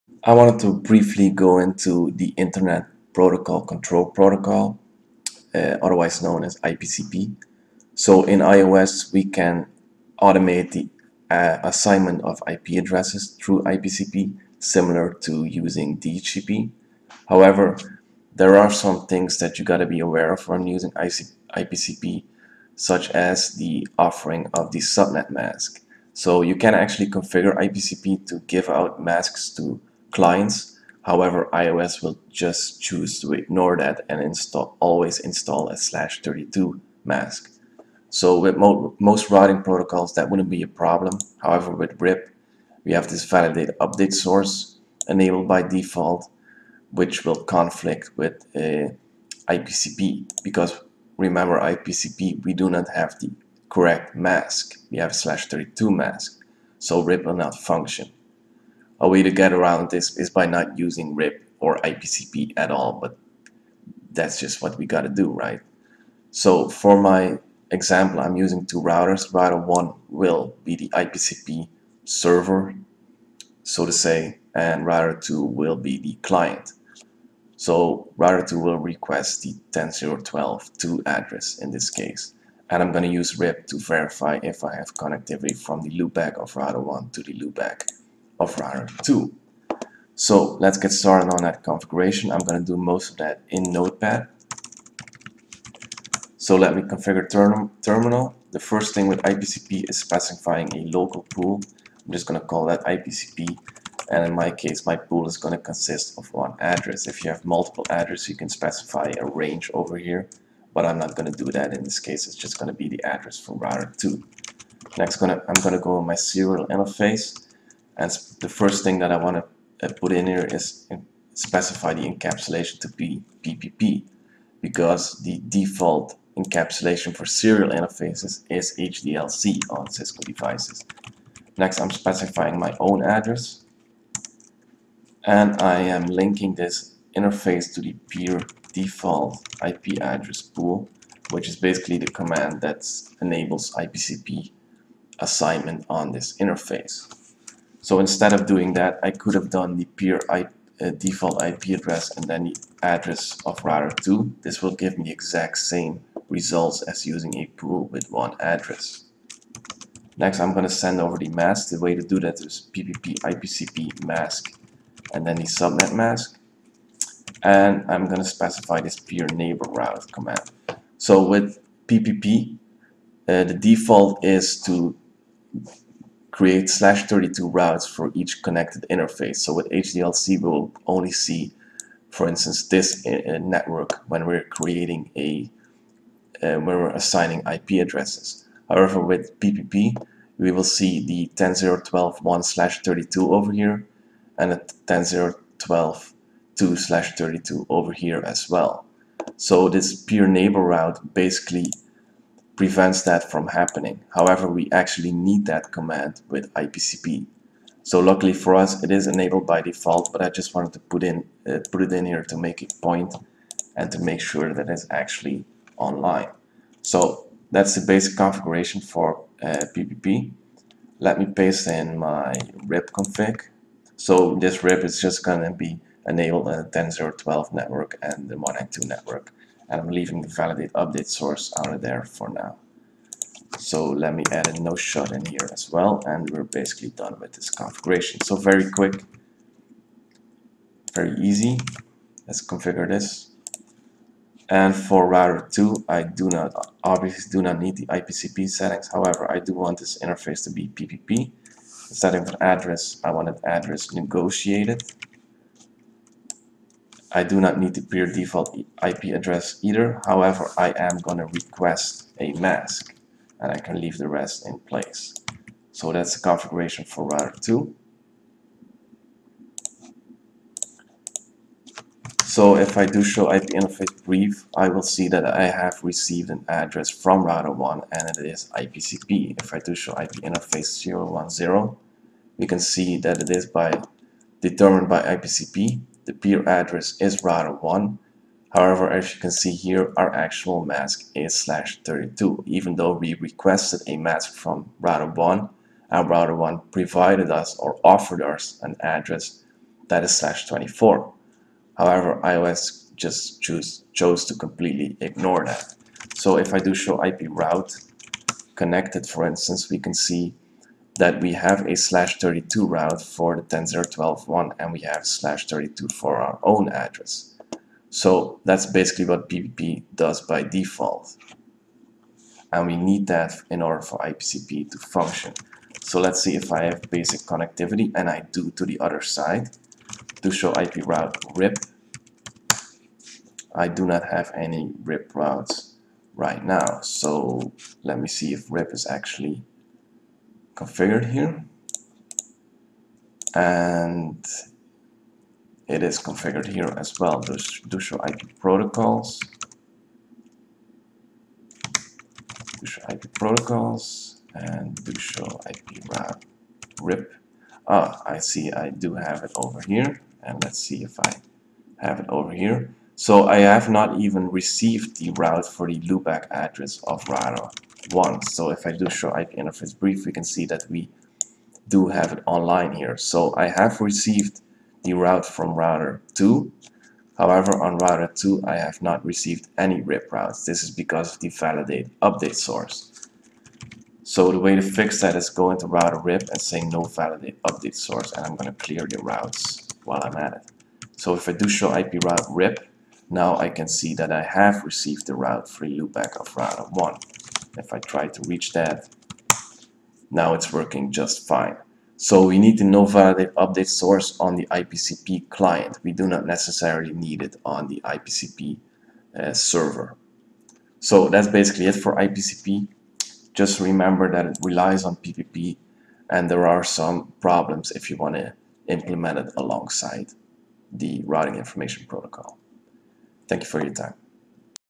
Quick Configs - IPCP (ppp, local pool, mask, rip)
This CCIE oriented episode of quick configs goes into configuring the Internet Protocol Control Protocol (IPCP). for all CCIE notes.